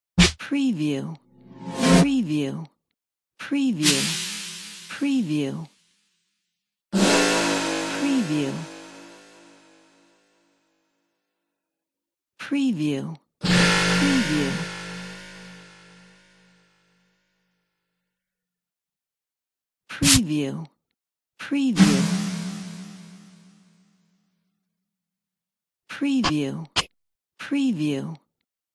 Trailer Swoosh SFX Pack Sound Effects
Cinematic Trailer Sound Design Pack (9 samples). High Quality, originality and dynamic transition or movement swoosh sfx.